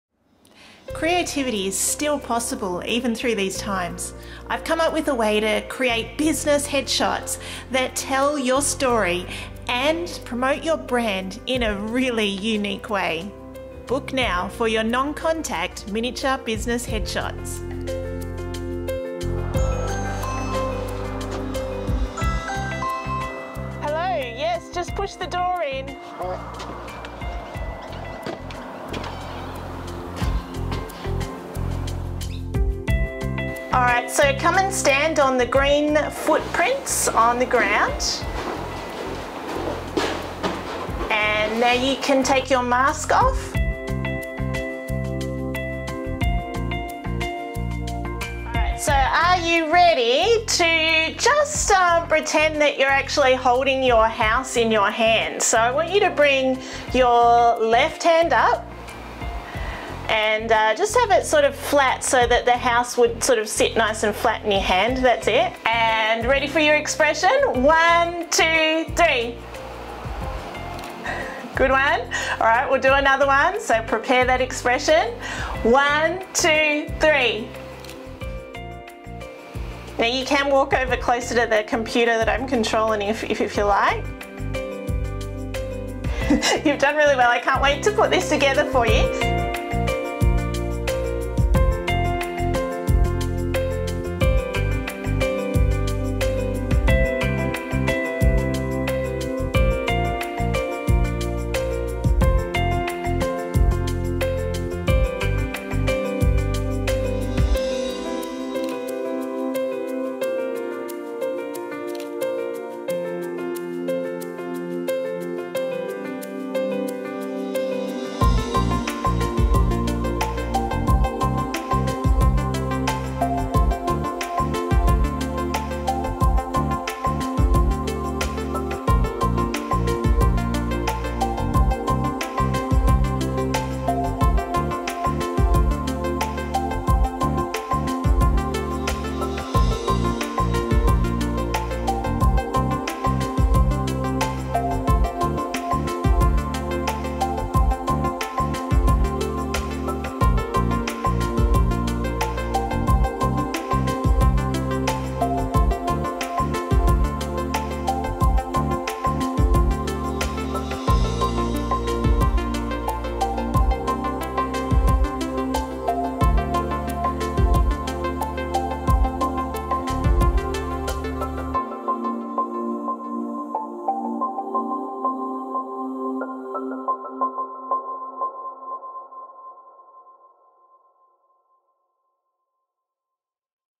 Business Miniature Headshots - Photography during Covid-19 lockdowns in Melbourne
During Covid-19 lockdowns, photographers have had to find new ways to be creative and offer services in a safe way.

Here at Story Art we have launched 'Business Miniature Headshots', a unique take on headshots that tell the story of your business through creative photography and photoshop.

This piece was created for Dragan, owner of who is a custom builder. Dragan has built his own exceptional residence in Upper Beaconsfield that showcases his craftmanship. This is his headshot... this tells his story...

Are you a cleaner, a landscaper, a hairdresser? Whatever your business, your story can be told creatively with a Story Art Business Miniature Headshot.

Even in Stage 3 lockdowns, business owners are able to travel for work. We are taking bookings from businesses within the Melbourne Metro area while in Stage 3 lockdowns in Melbourne. The session is completely non contact, you will be the only person in the main studio during your session, and I will communicate with you through a video camera and will control my camera via tethering and remote triggering.